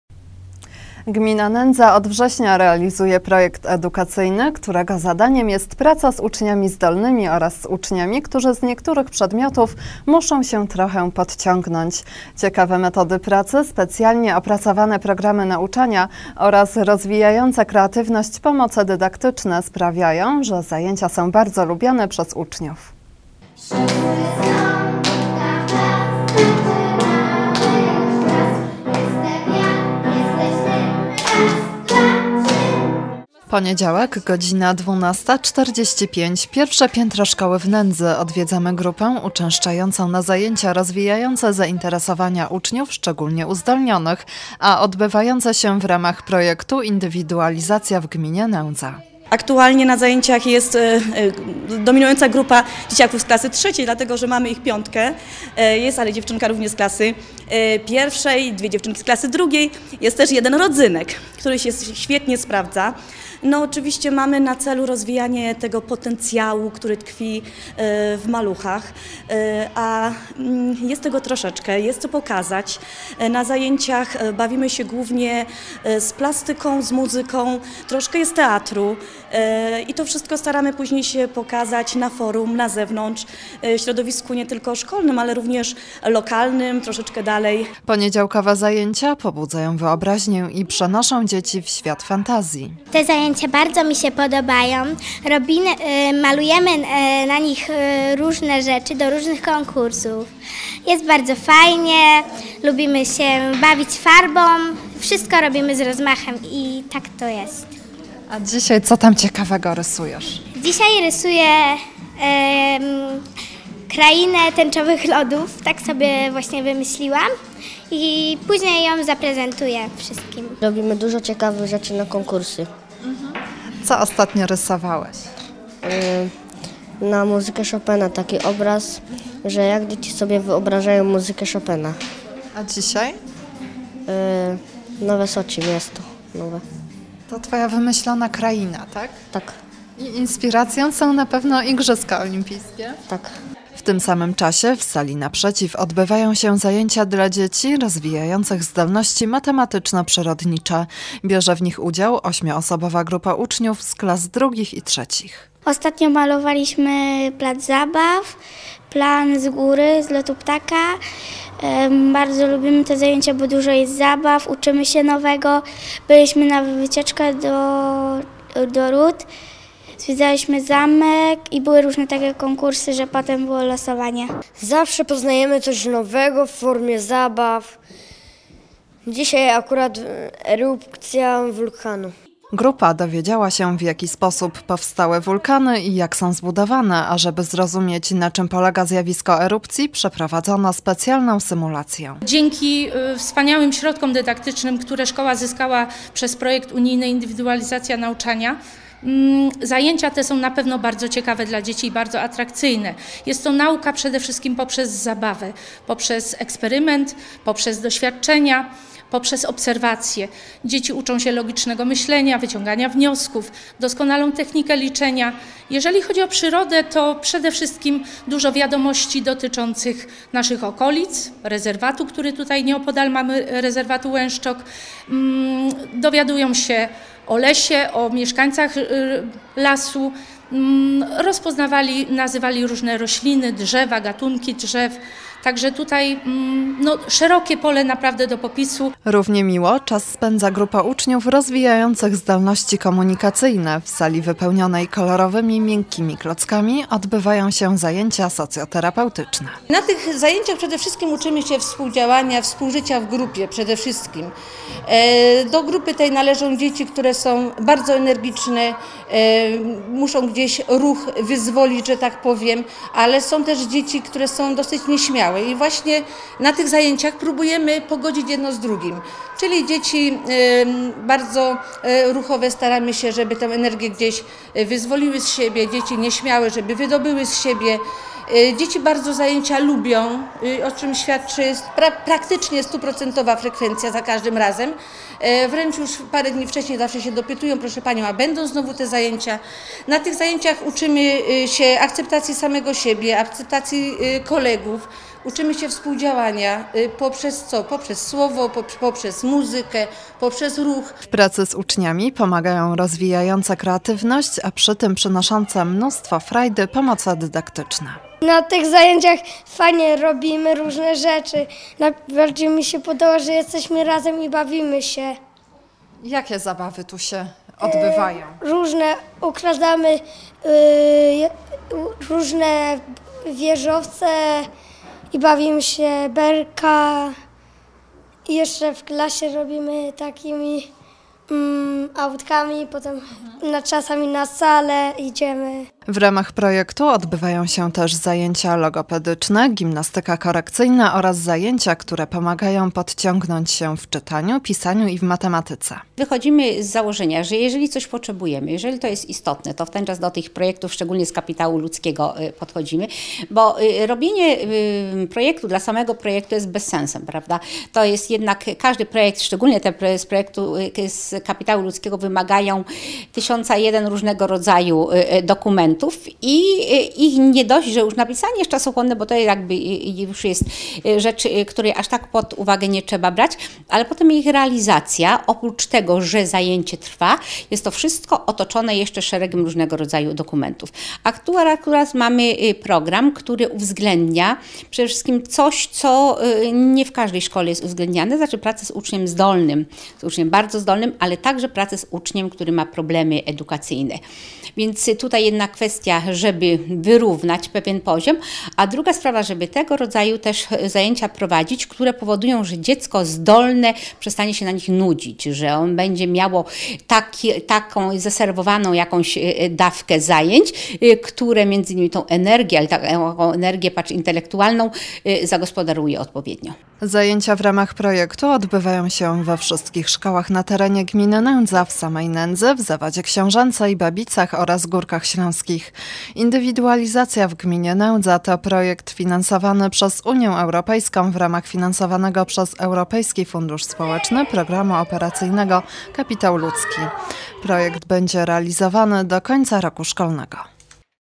Z indywidualnym podejściem do ucznia w Nędzy
Ponad 80 uczniów z klas I-III objętych jest indywidualnym kształceniem w gminie Nędza. W czterech szkołach w ramach projektu "Indywidualizacja w gminie Nędza" dzieci biorą udział w specjalnych zajęciach w których wykorzystuje się atrakcyjne pomoce dydaktyczne.